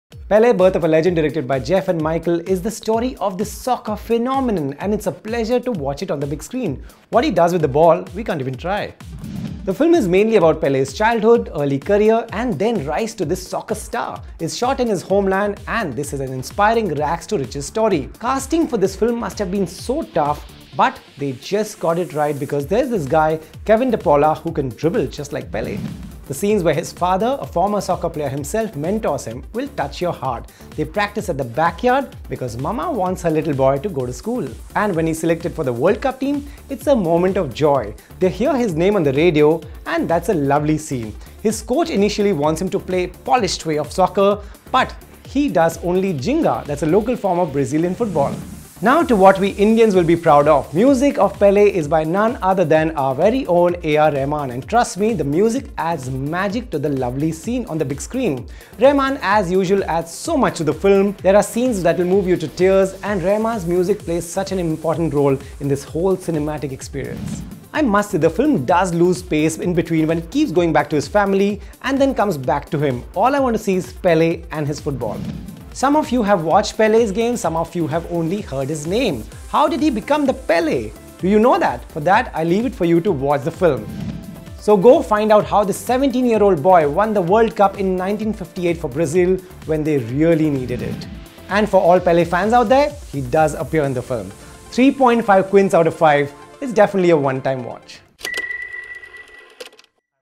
Movie Review - Pele: Birth of a Legend
Movie review of the Pele biopic. Directed by Jeff Zimbalist and Michael Zimbalist. Music is by A.R.Rahman